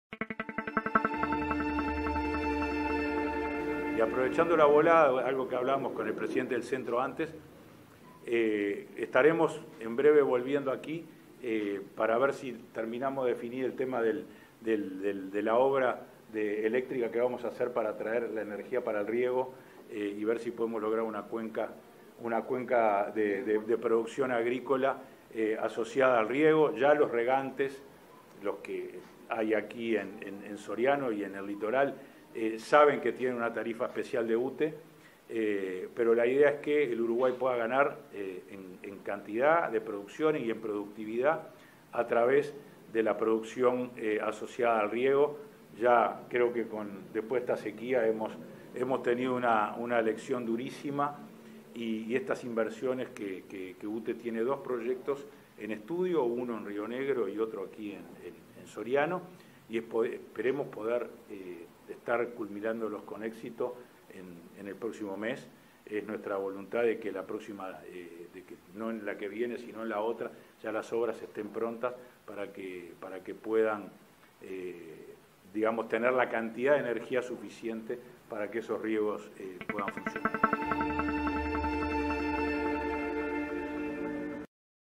Julio Luis Sanguinetti, vicepresidente de UTE
En las próximas semanas UTE definirá las características  del programa de riego intensivo para Soriano